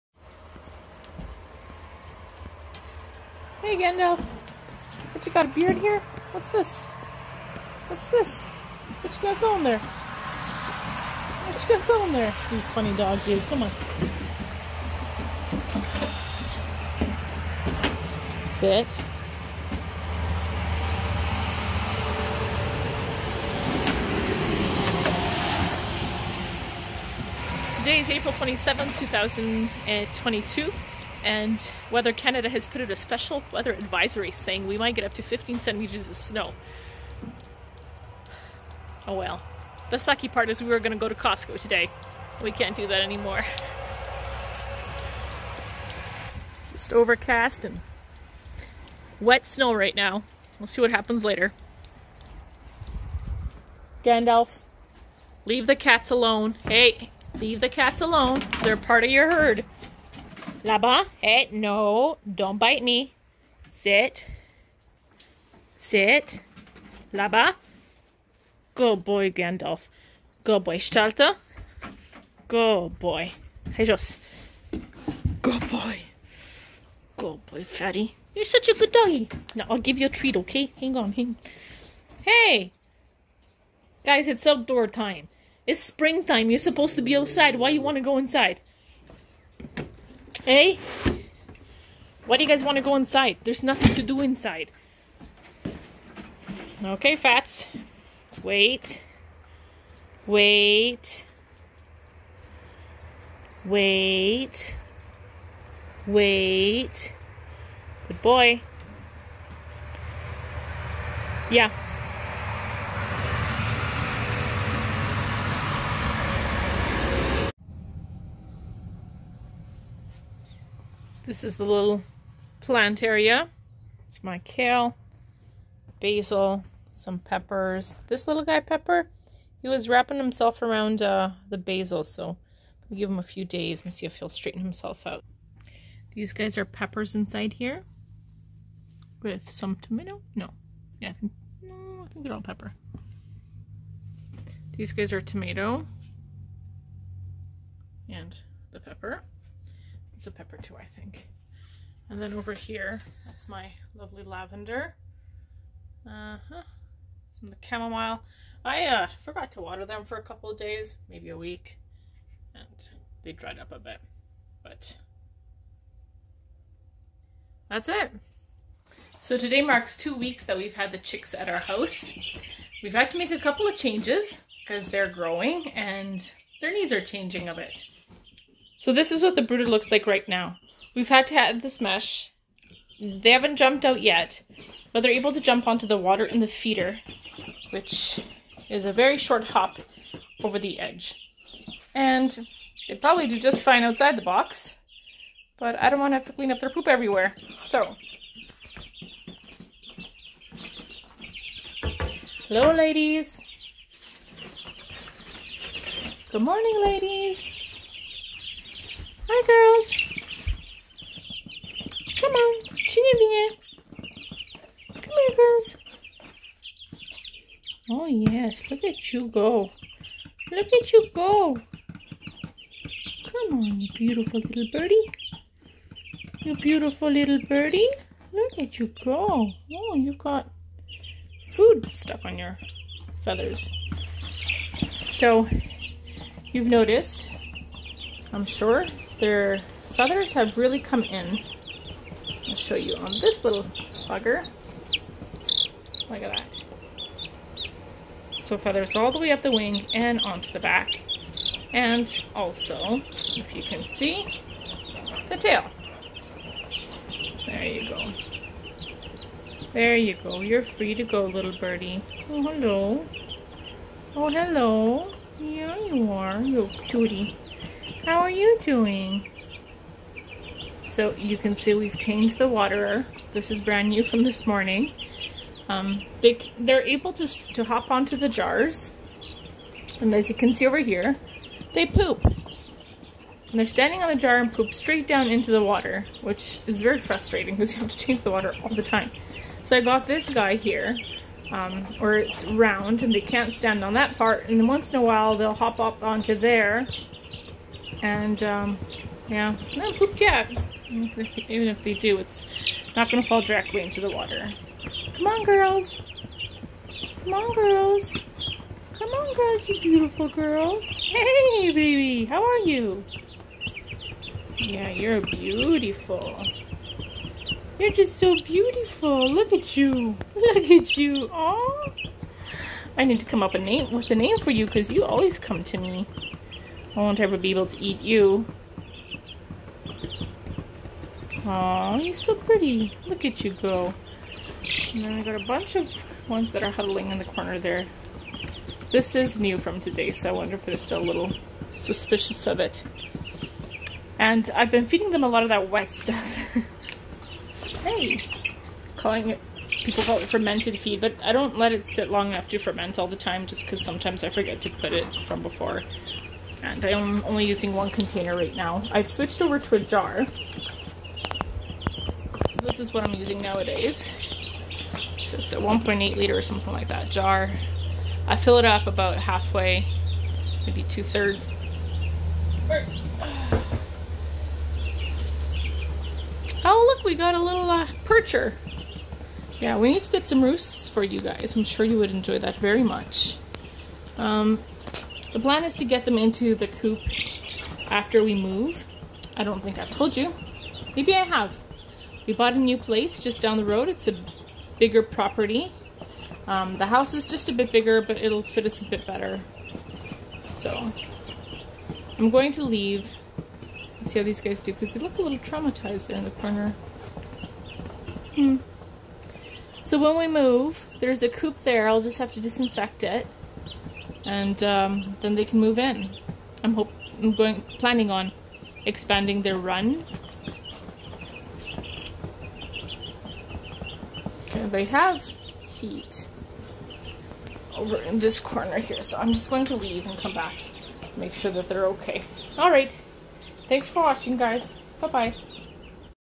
Gandalf the Great Pyrenees, April Weather, and Our Chicks At 2 Weeks.
Although the calander says it's spring, the weather in our area says spring is still around the corner. Gandalf is getting better at his manners and our little ladies are growing up!